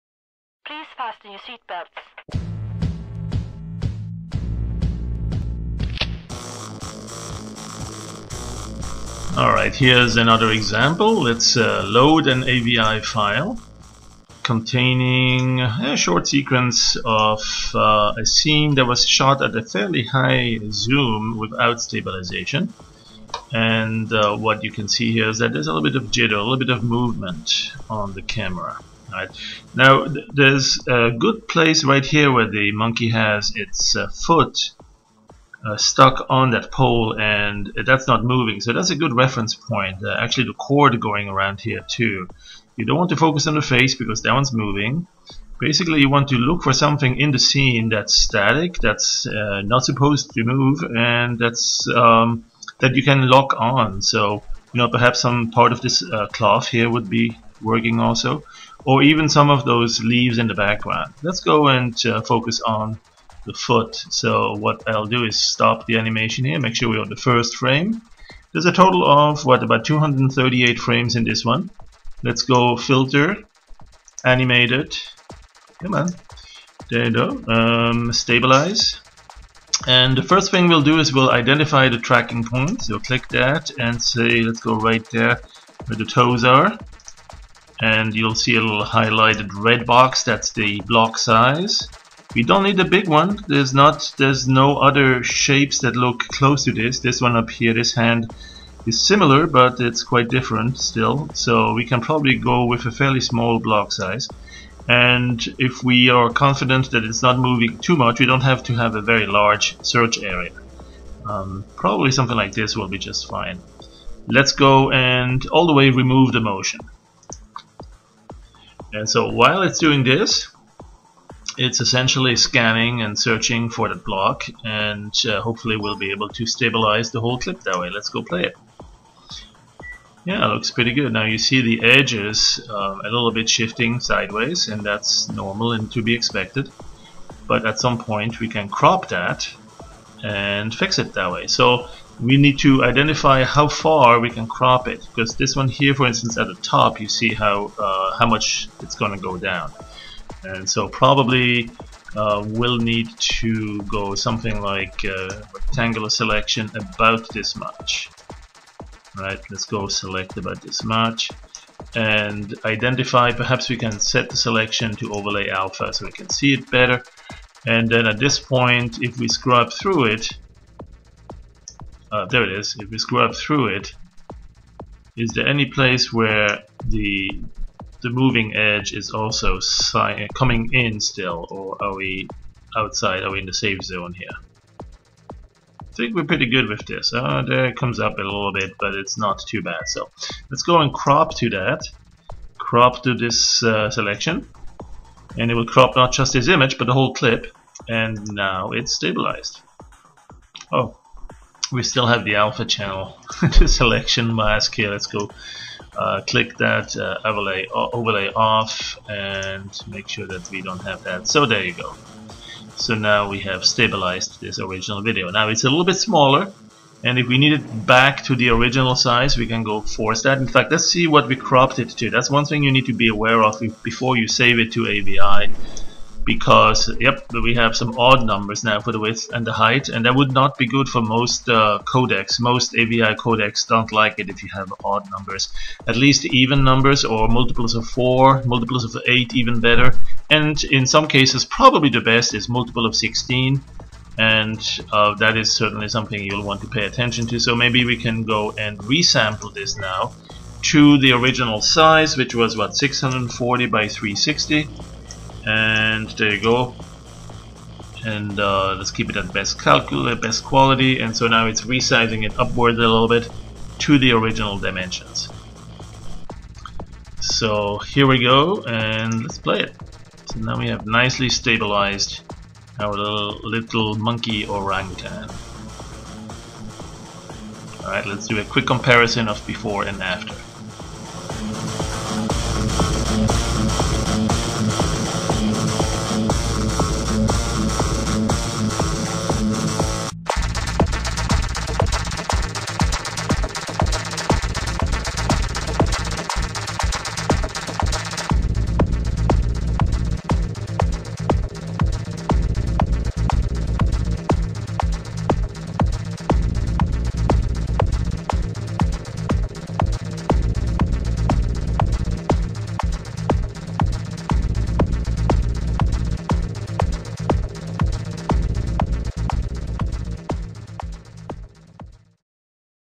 Image stabilizer - Taming the Monkey
PD Pro 7.1 has a new image stabilizer in the Animated Filters category.